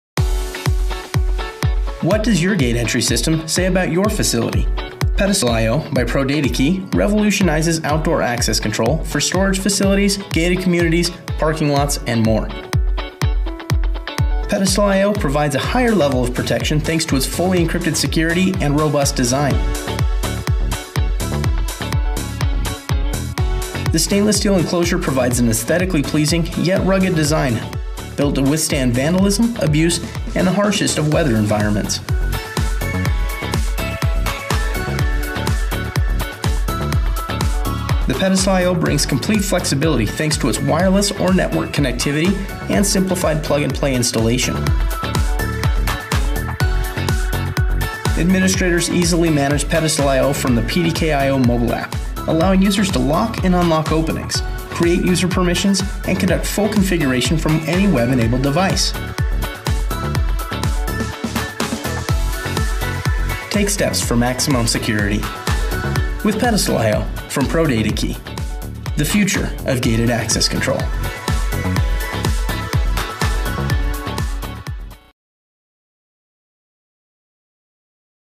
Gate Access Card Reader For Parking Lots, Storage Facilities, and Gated Communities
The Gate Access Card reader is the perfect weatherproof solution for read-in and read-out access control in parking lots, gated communities, and storage facilities. Its stainless steel housing, 10-key input, and a built-in card reader make it the most versatile plug n’ play outdoor controller on the market.

If you are looking to protect your properties and have it still look good, this is the reader for you. It can be used for gated communities, storage facilities, and parking lots to provide extra security while still looking good.

It can be wireless or wired depending on what you want making this highly versatile for your needs.